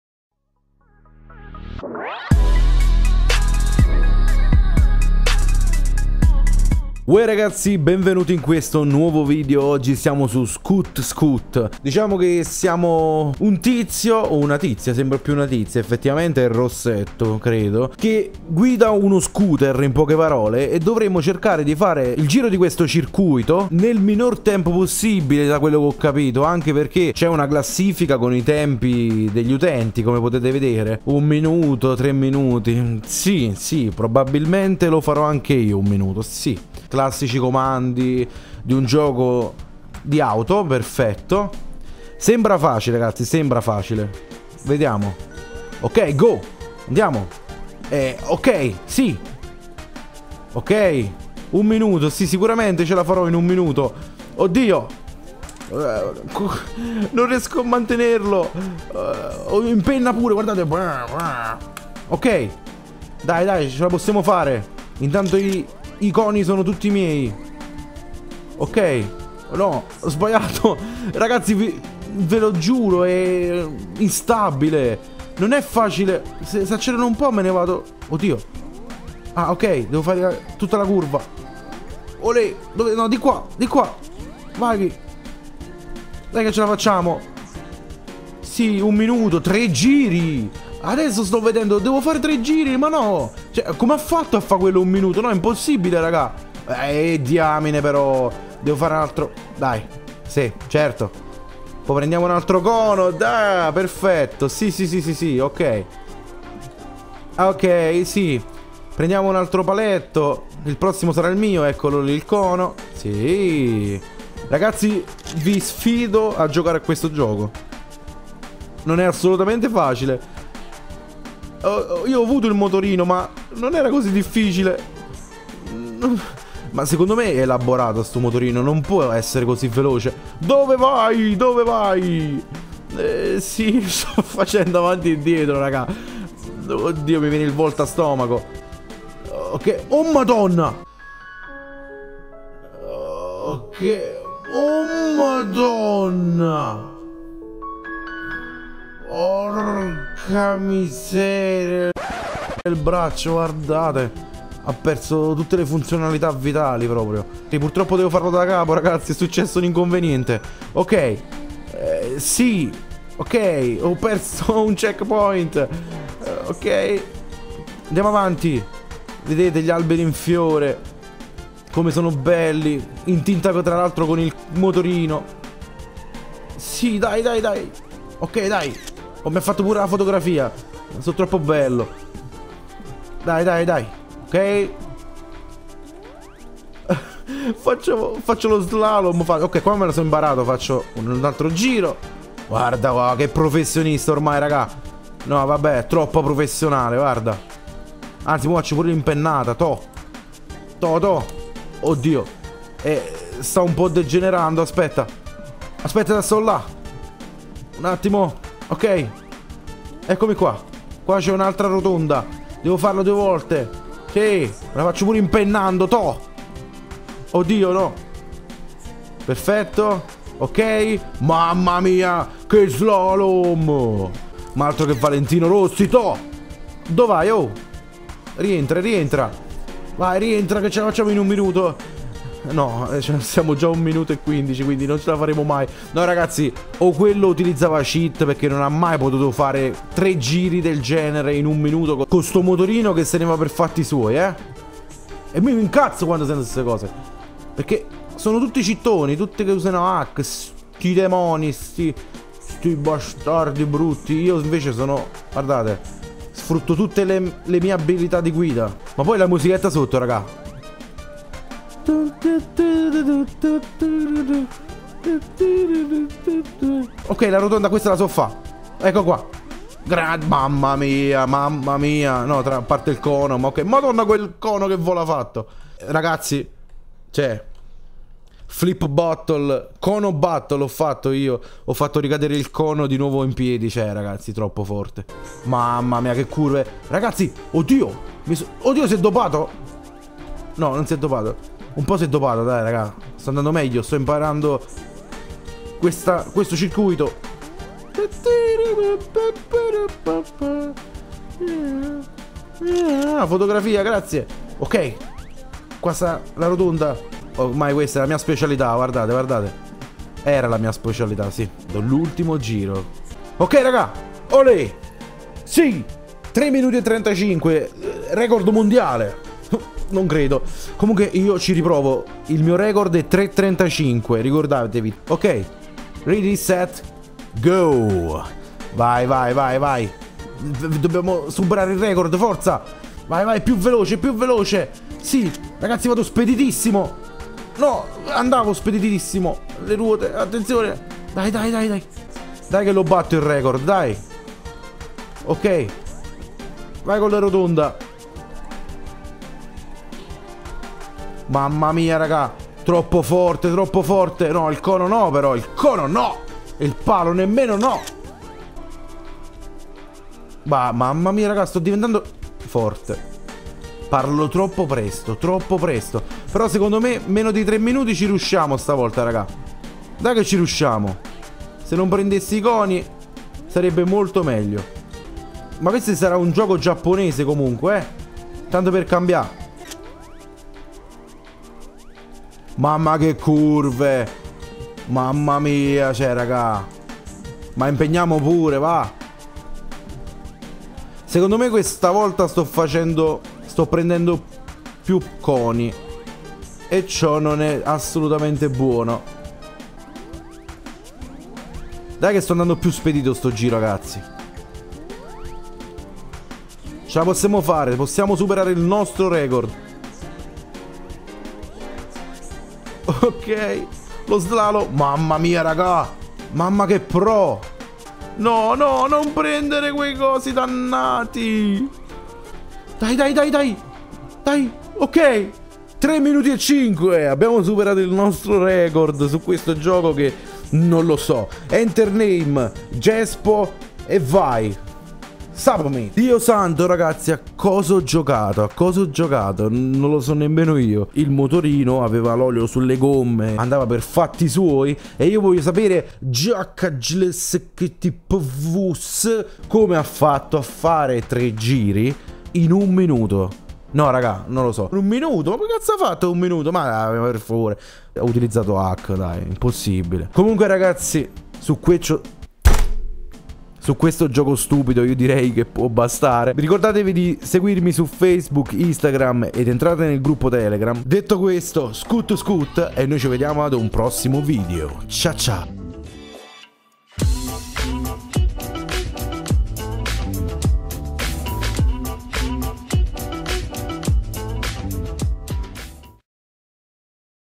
COME GUIDARE UNO SCOOTER FUORI CONTROLLO!
Uno gioco FIGO che ti insegna SICURAMENTE come guidare uno scooter, sì.. Se il video è stato di vostro gradimento, mettete un like, iscrivetevi al canale e non dimenticatevi di seguirmi su tutti i miei Social!

Cià cià